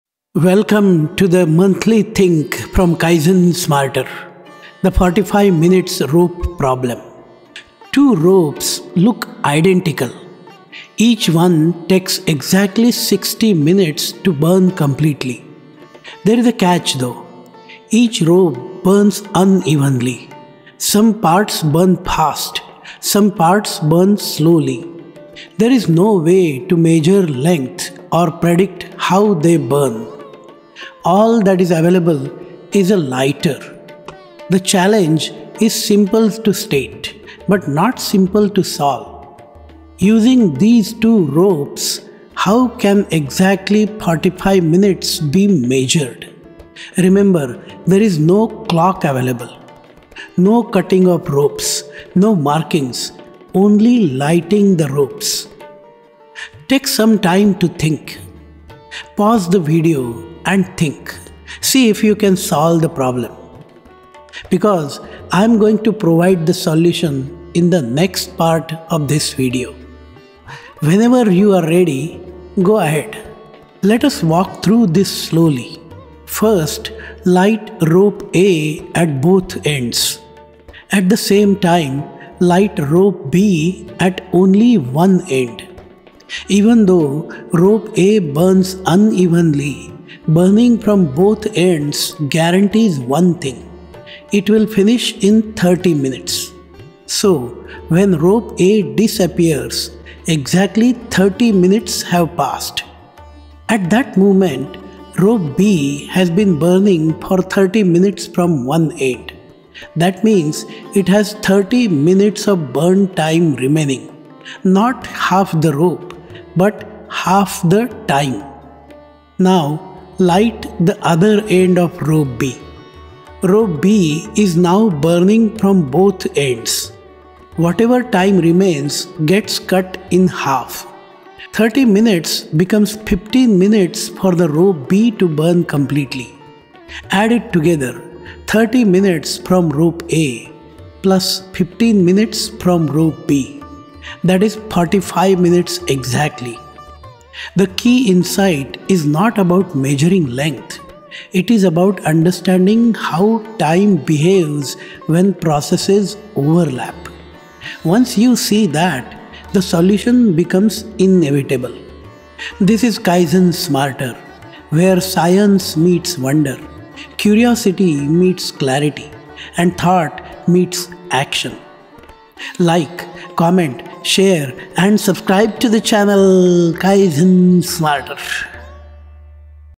How to Solve the Two Ropes Puzzle to Measure 45 Minutes
Two identical ropes each burn for 60 minutes, but unevenly.
Using only a lighter, how can exactly 45 minutes be measured?

This video walks through the problem calmly and explains the solution step by step - focusing not just on the answer, but on how to think about the puzzle.

If you love understanding the mind, improving yourself, and seeing the world with new eyes, this channel is for you.